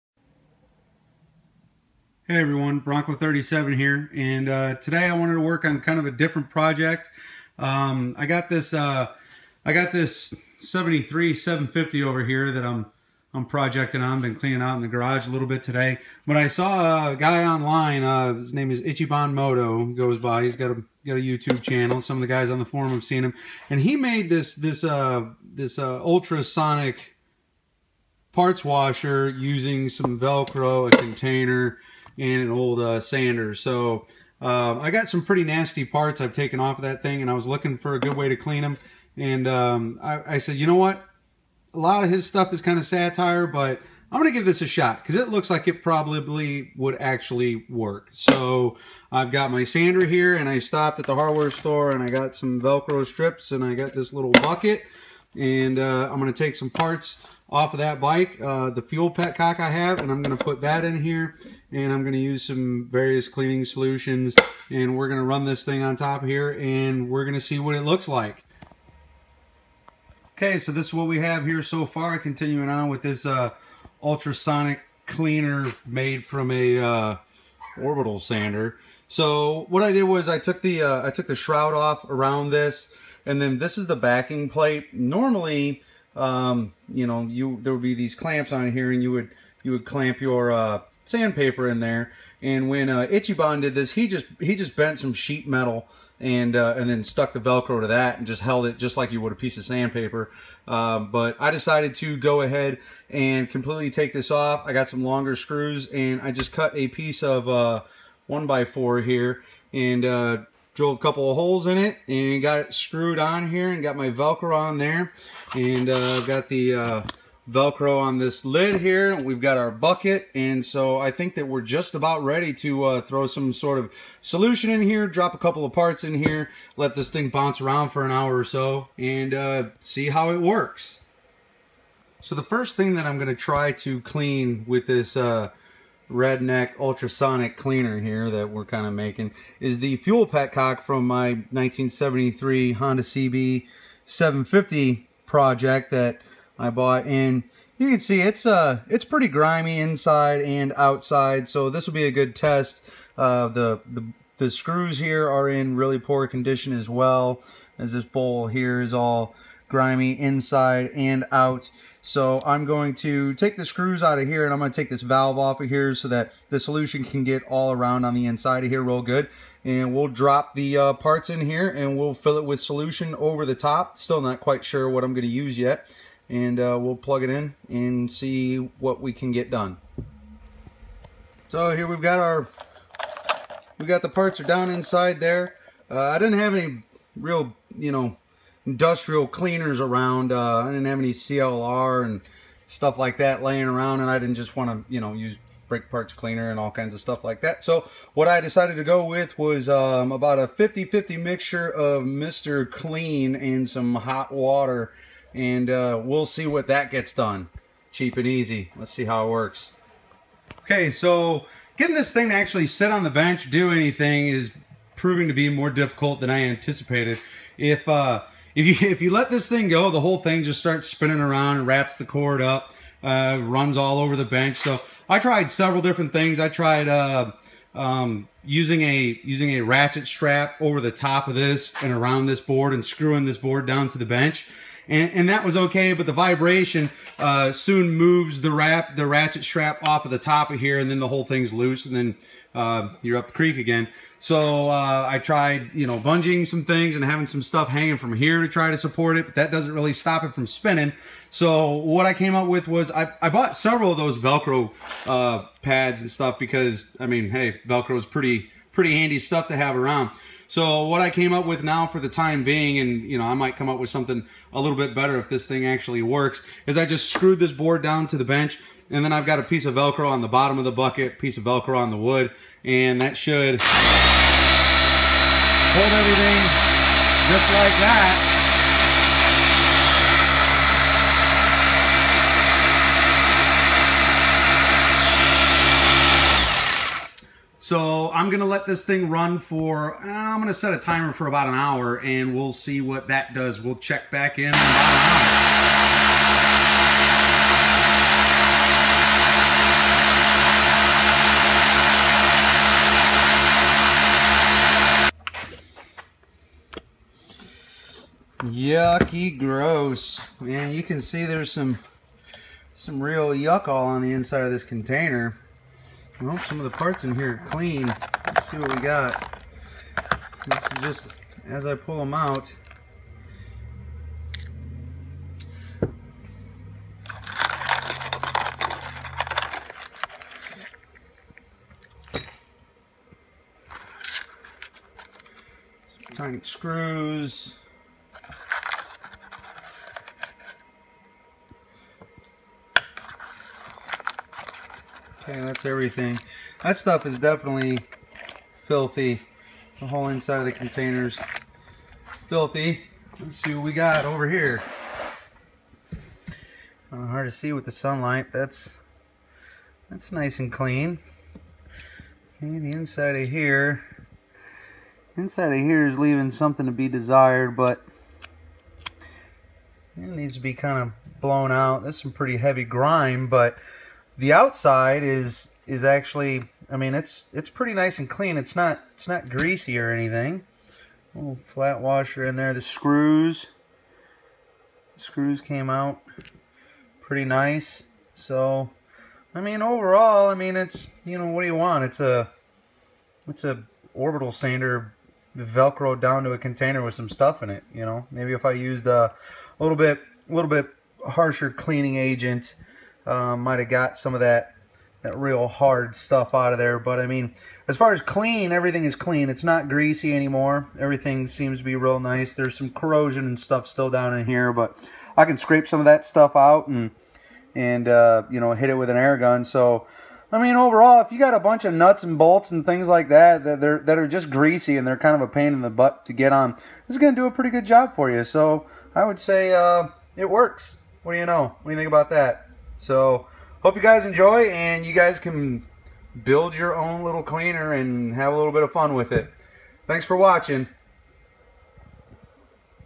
Home Made Ultra Sonic Cleaner
I saw Ichiban Moto make this and I wanted to check and see for myself if it works. I believe it works great. A very easy way to clean and de-grease small parts for many applications.